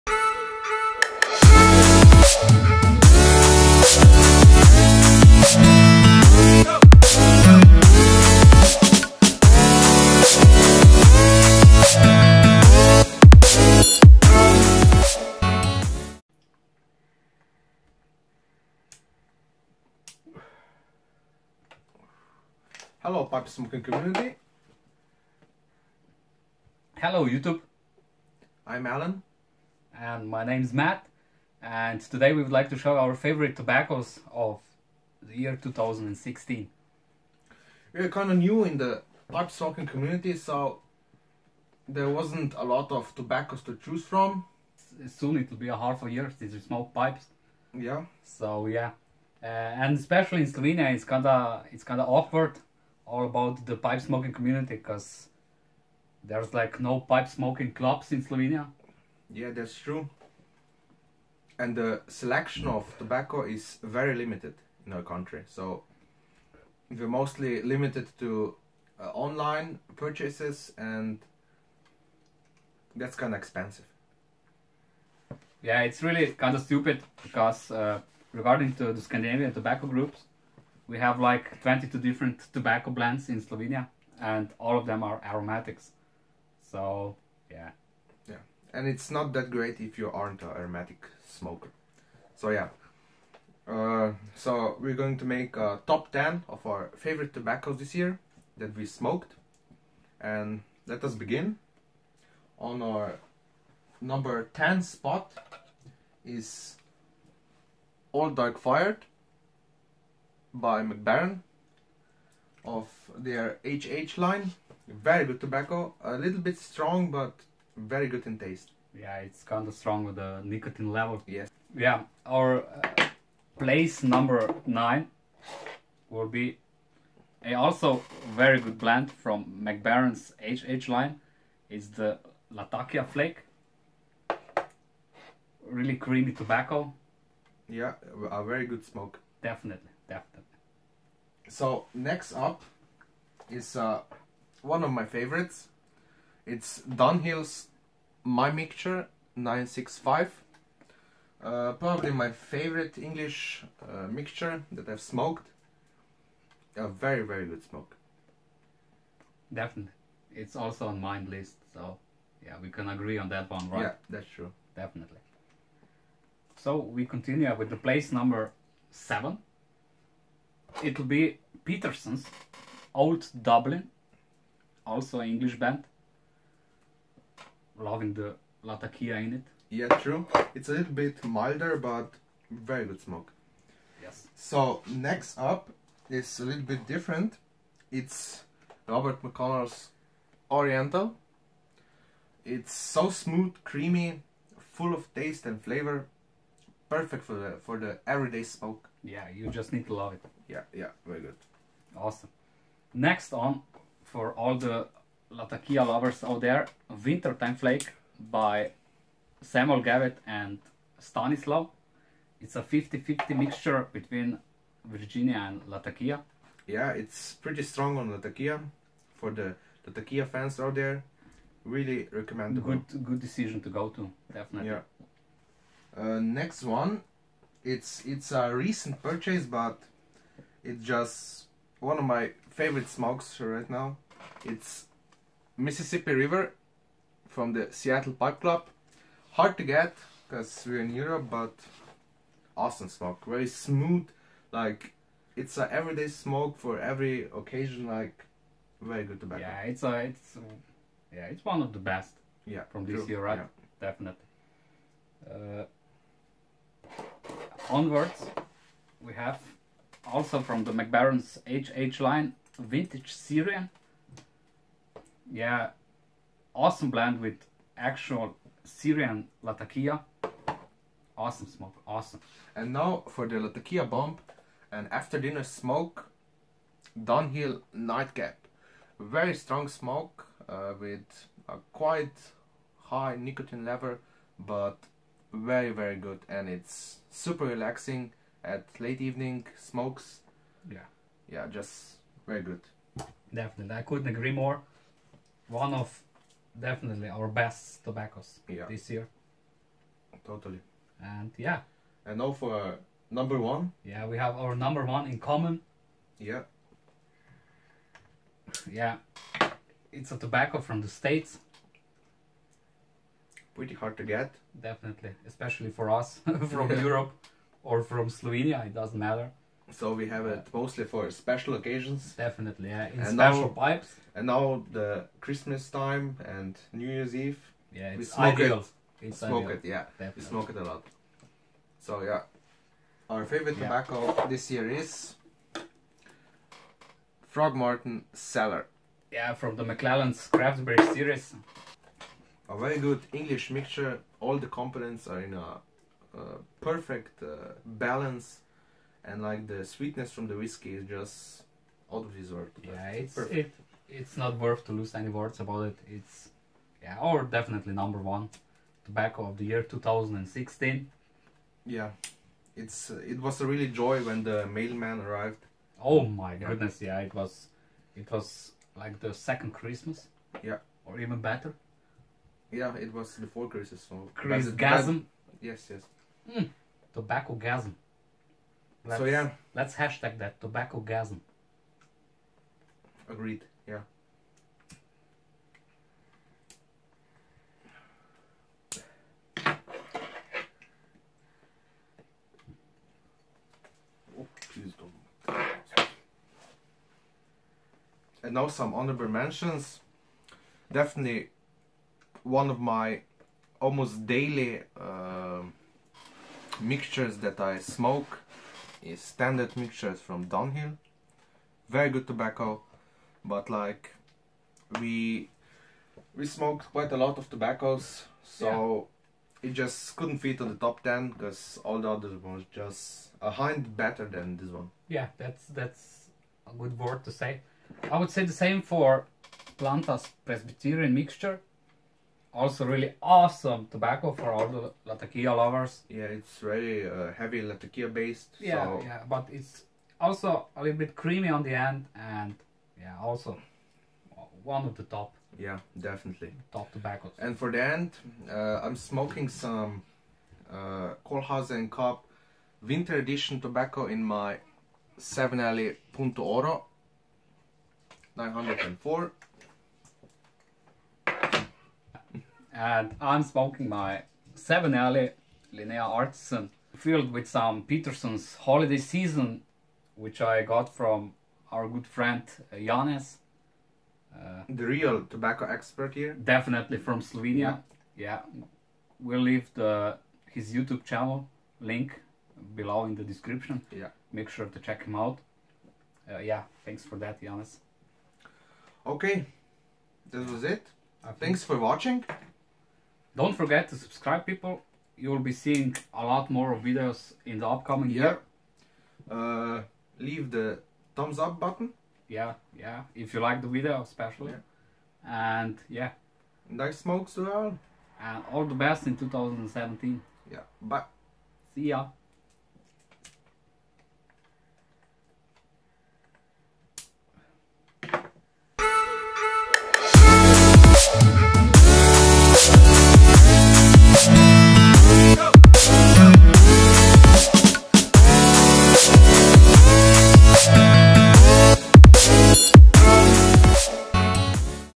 Hooked Pipers - Our finest tobaccos of 2016
We show you our finest tobacco blends of the year 2016 that we had the opportunity to smoke.